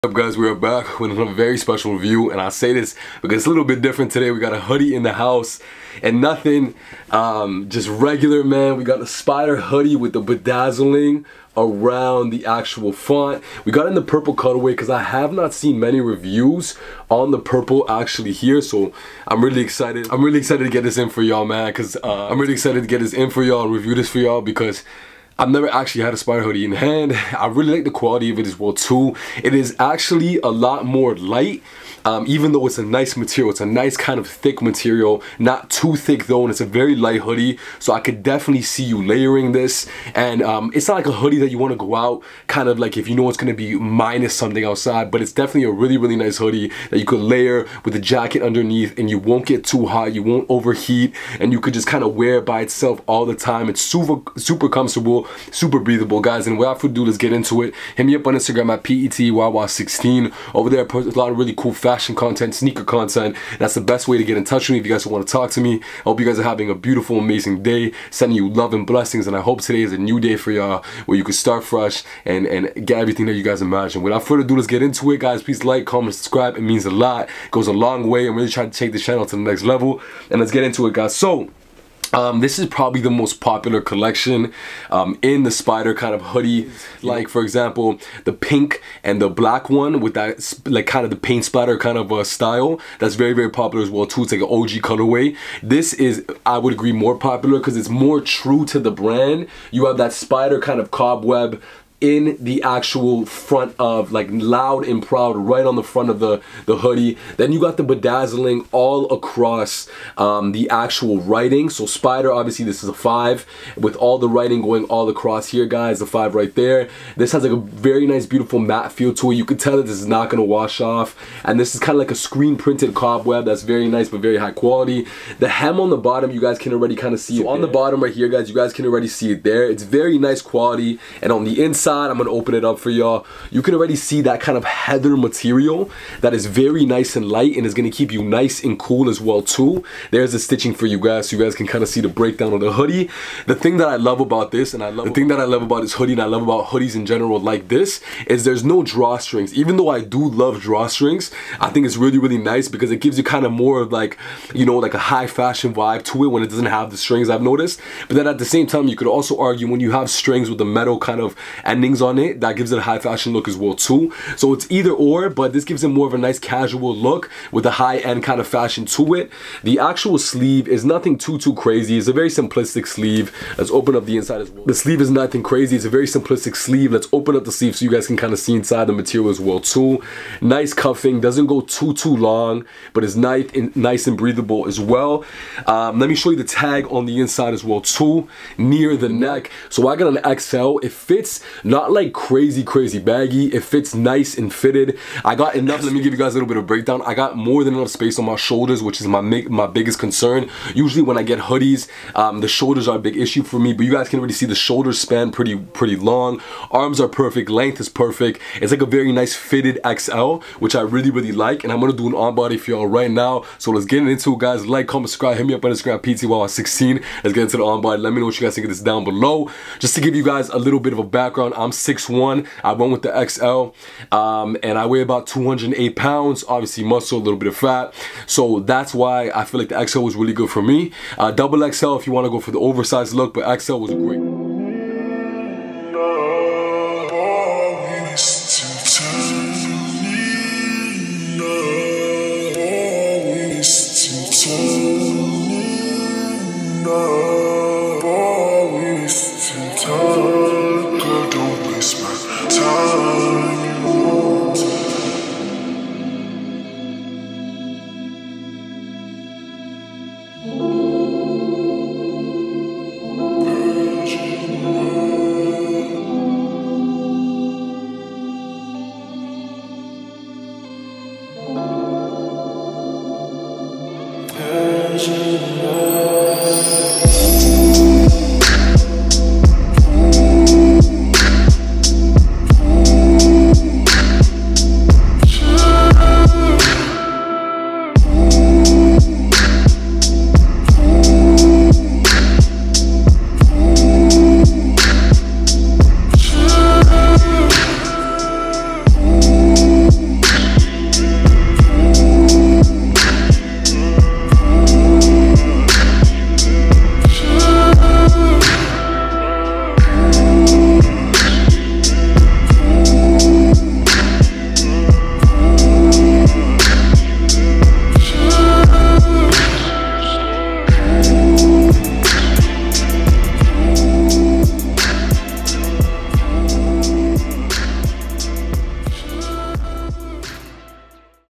SP5DER Web Purple Hoodie (Review) + ON Body
YURRRRR. This brand has blown up due to Young Thugs influence, and the recent trend of coloured tracksuits with vibrant screen printed and popping logos. SP5DER, the streetwear clothing brand curated by Young Thug, stands as a testament to the artist's influence on both the music and fashion worlds.

With its bold designs, fusion of street and luxury, and commitment to individuality, the brand encapsulates the essence of urban culture and represents the spirit of self-expression for a generation unafraid to push boundaries and embrace their own unique style.

web: www(dot)coolkicksmall(dot)com
code: petey

LOVE U.